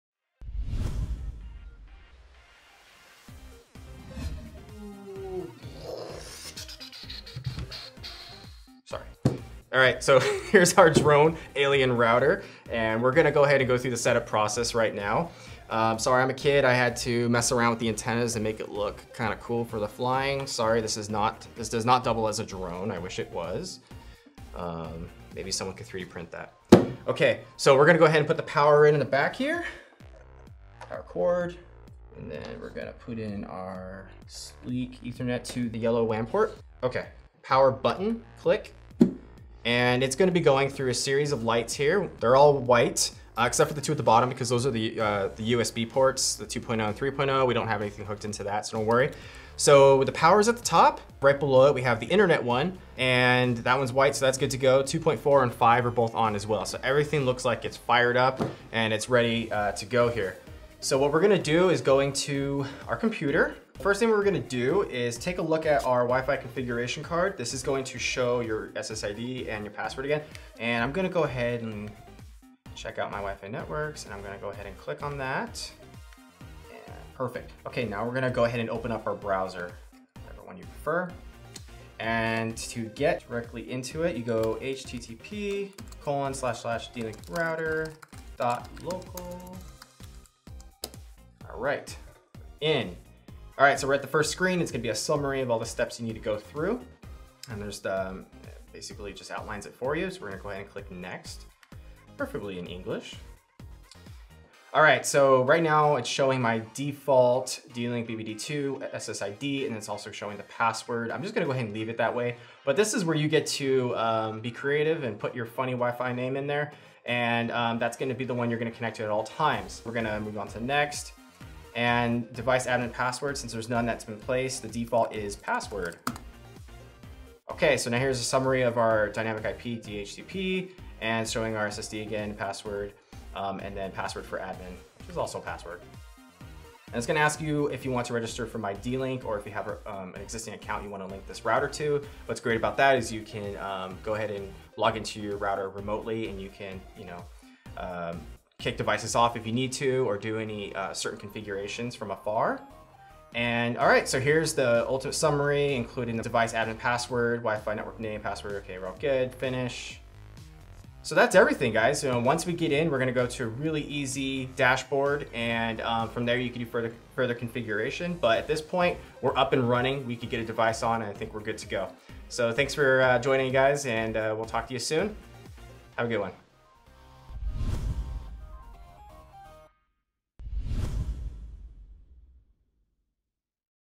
How to Setup the AC3200 Ultra Wi-Fi Router
Check out this video for step by step setup instructions for the AC3200 Ultra Wi-Fi Router (DIR-890L/R).

for tips & tricks on the ultimate connected home